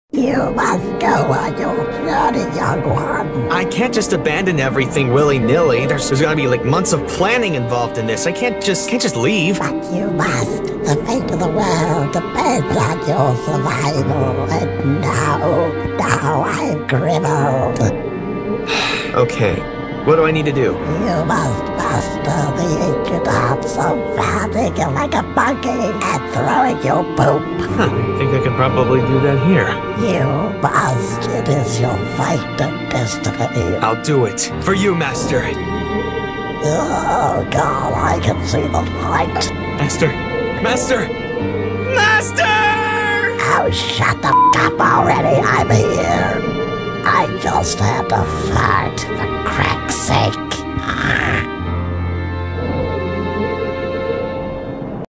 Counsel from the Master Goat
I don't really know. This just happened in a skype call with my friend, Dane Spurrell.